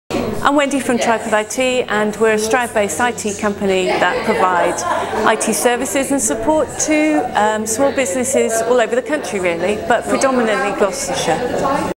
Wendy Clifford - Tripod-IT
Professional computer and network support

Tripod IT are a Microsoft Certified Small Business Specialist offering computer and network support to businesses in Gloucestershire.

King's Daughters Women in Business offers it's members short promotional videos.